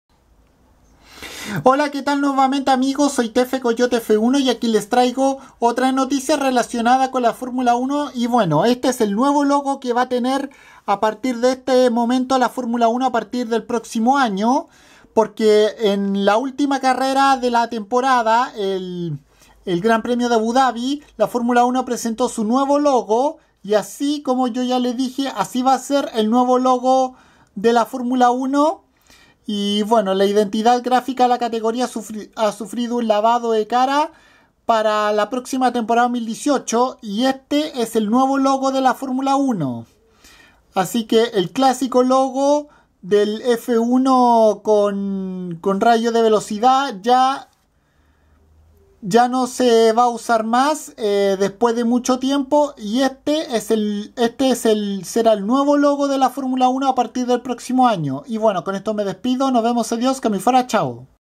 Este es el nuevo logo de la Fórmula 1
En el reciente GP de Abu Dhabi se presentó el nuevo logotipo de la F1 que estará a partir del próximo año